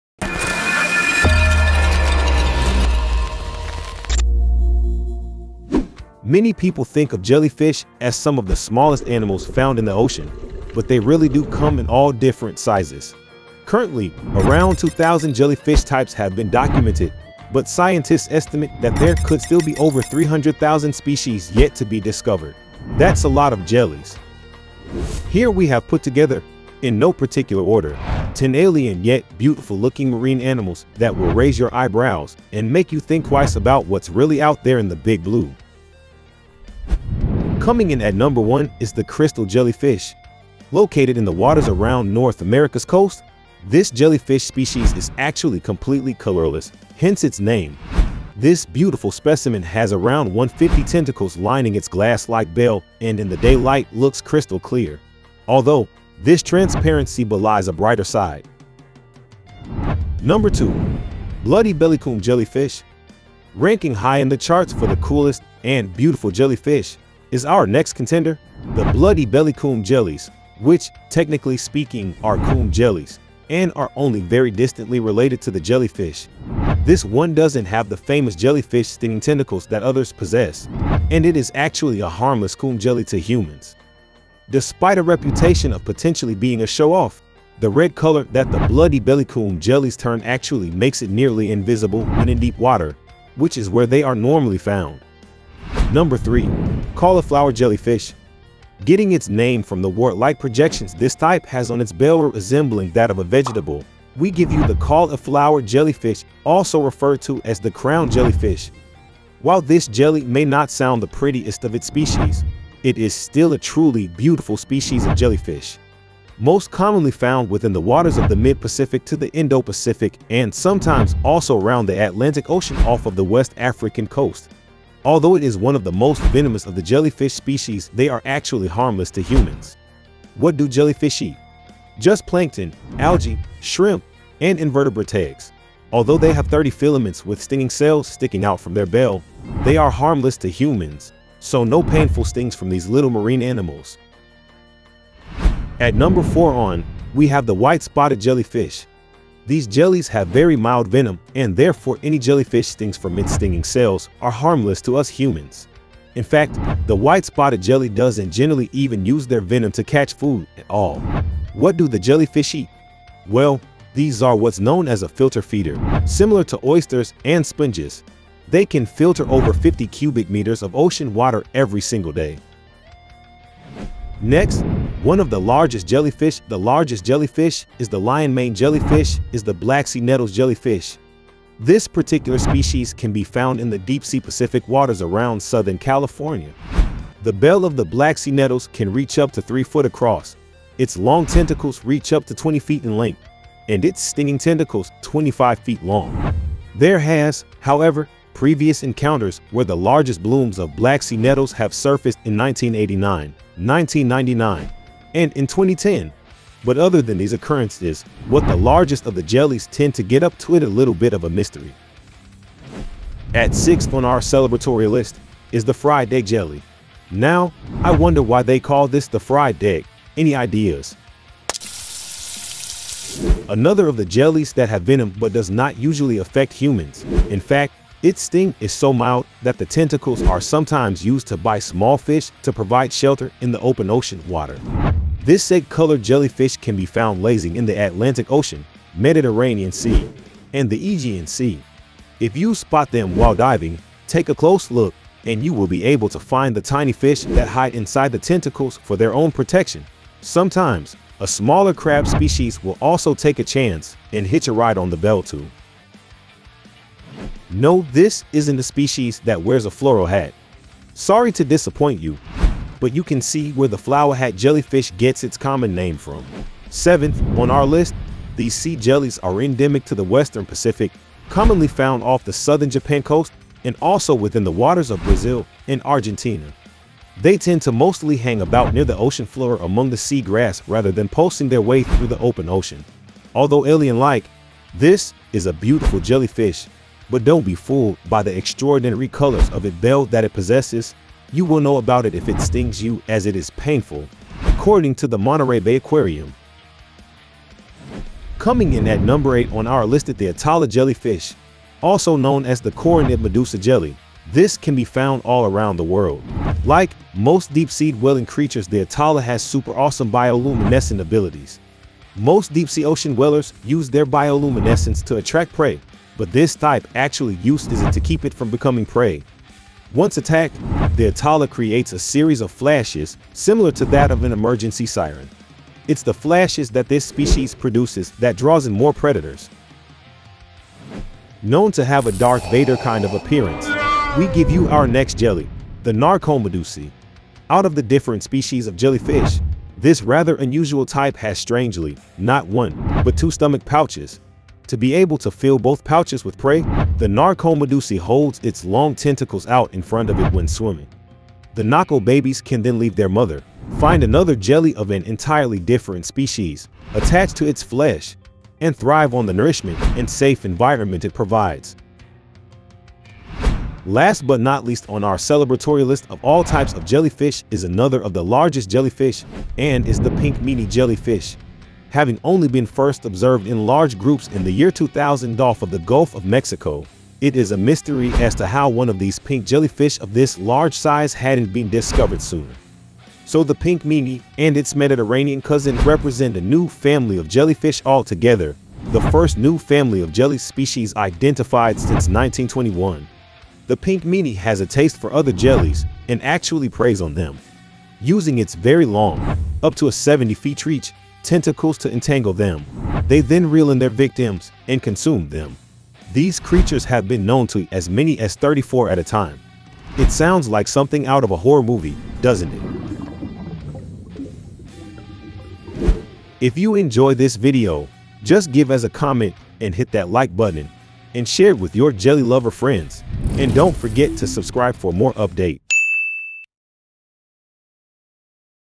10 Different Types of Jellyfish
Did you know over 2000 species of jellyfish live in the world's oceans? In this video, we'll explore 10 different types of jellyfish that make our seas so vibrant and diverse. You won't believe some of their unique shapes and colors! Learn more about these graceful ocean creatures now.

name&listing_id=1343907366